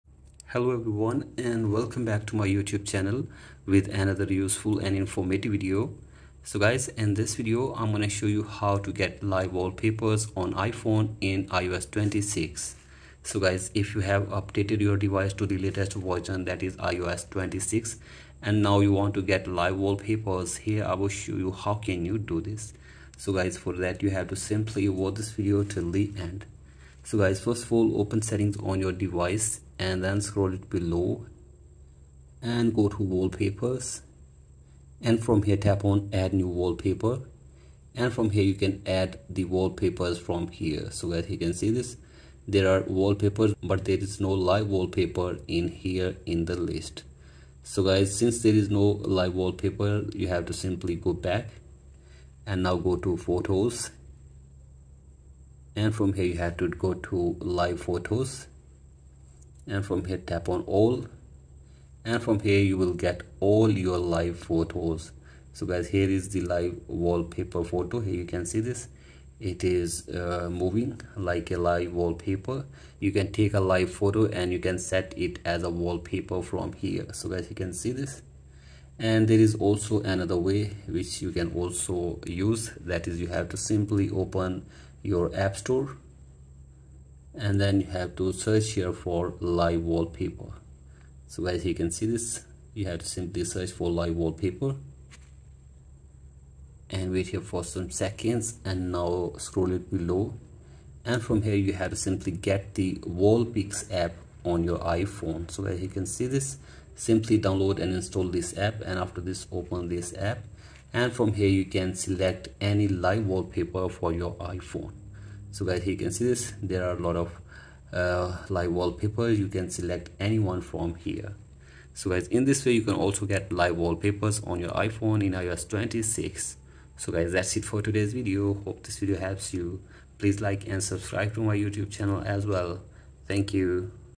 How to Get Live Wallpapers on iPhone / iOS 26
Want to bring your iPhone home screen to life? In this video, I’ll show you how to get and set live wallpapers on your iPhone running iOS 26 — whether you're using dynamic wallpapers, Live Photos, or third-party apps!